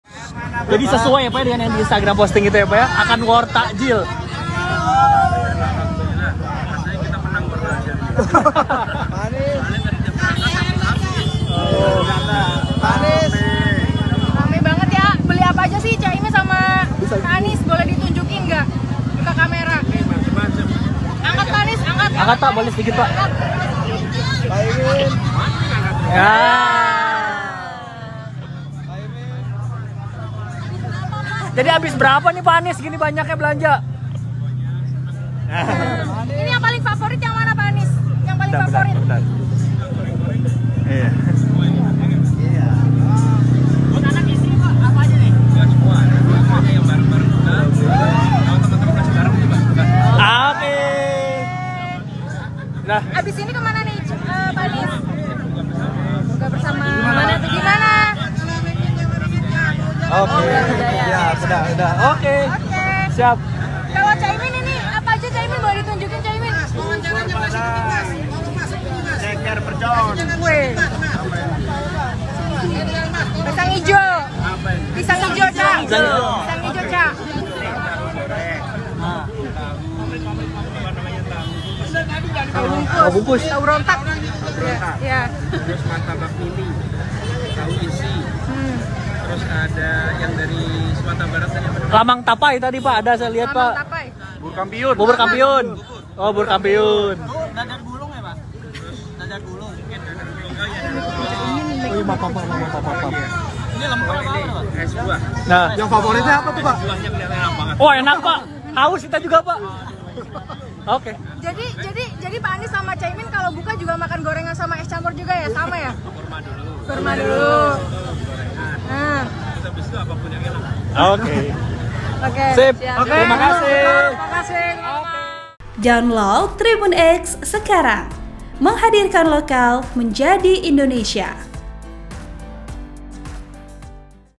Jelang KPU Umumkan Hasil Pilpres 2024, Sorotan Reaksi Santai Anies-Cak Imin Semringah 'War' Takjil
TRIBUNJATIM.COM - Pasangan Anies Baswedan-Muhaimin Iskandar berburu takjil jelang buka puasa pada Rabu (20/3).

Alih-alih tegang menunggu pengumuman resmi KPU soal hasil Pilpres 2024, Anies-Cak Imin justru memakai baju couple.

Keduanya semringah saat berhasil membawa beberapa kantong plastik berisi takjil.